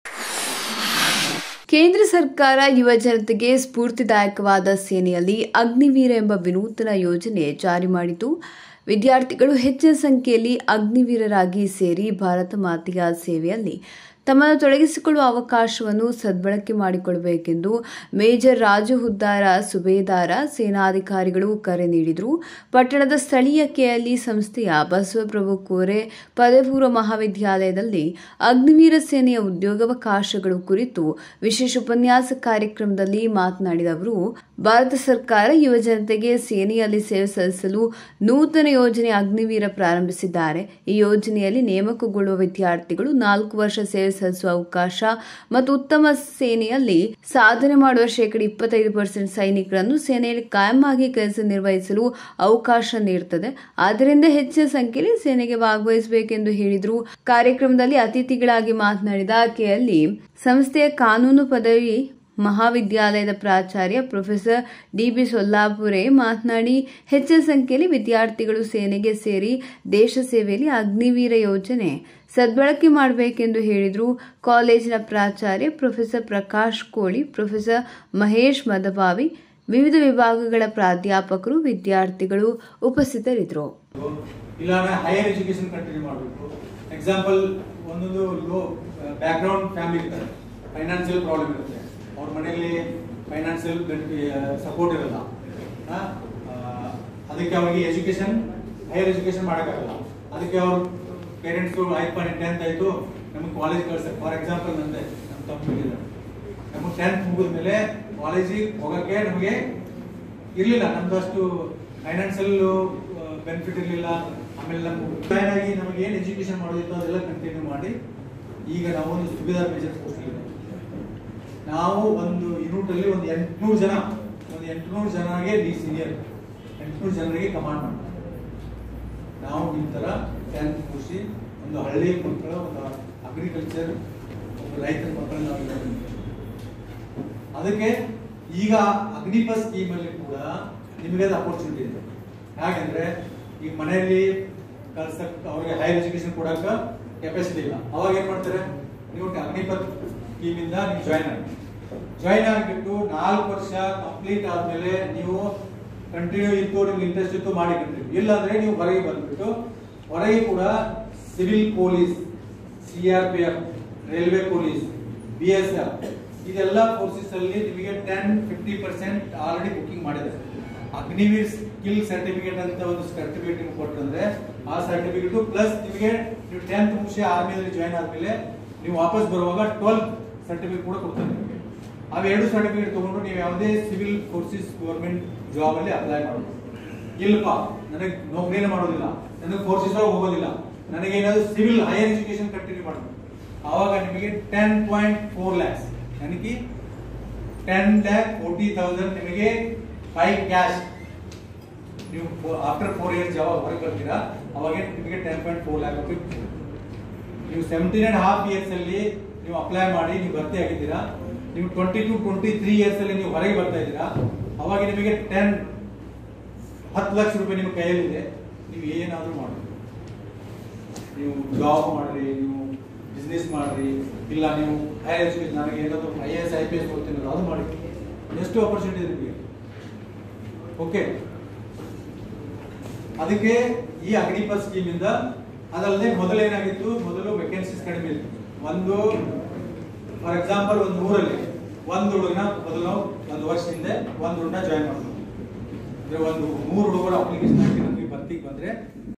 ಚಿಕ್ಕೋಡಿ : ಅಗ್ನಿವೀರ ಸೇನೆಯ ಉದ್ಯೋಗವಕಾಶಗಳ ಕುರಿತು ವಿಶೇಷ ಉಪನ್ಯಾಸ
ಚಿಕ್ಕೋಡಿ :ಬಸವ ಪ್ರಭು ಕೋರೆ ಪದವಿಪೂರ್ವ ಮಹಾವಿದ್ಯಾಲಯದಲ್ಲಿ ಅಗ್ನಿವೀರ ಸೇನೆಯ ಉದ್ಯೋಗವಕಾಶಗಳ ಕುರಿತು ವಿಶೇಷ ಉಪನ್ಯಾಸ (ICB NEWS CHIKODI)